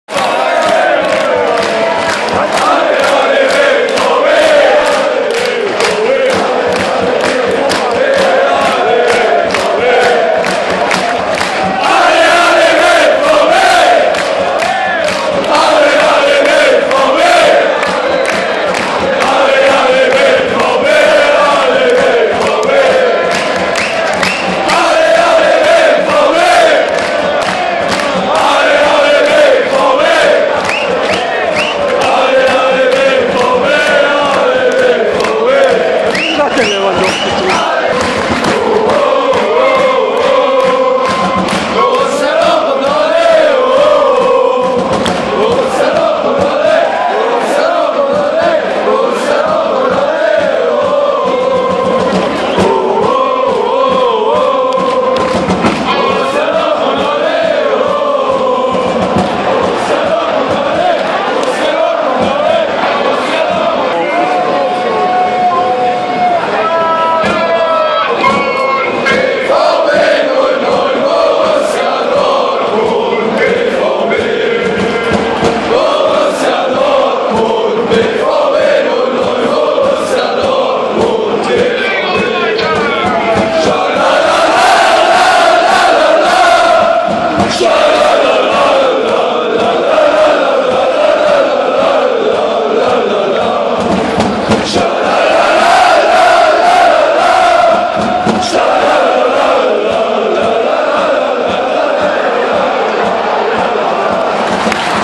Bremen - Dortmund (1:3) BVB Fans Stimmung auswärts!
Werder Bremen - Borussia Dortmund (1:3)
BVB Fans Stimmung in Bremen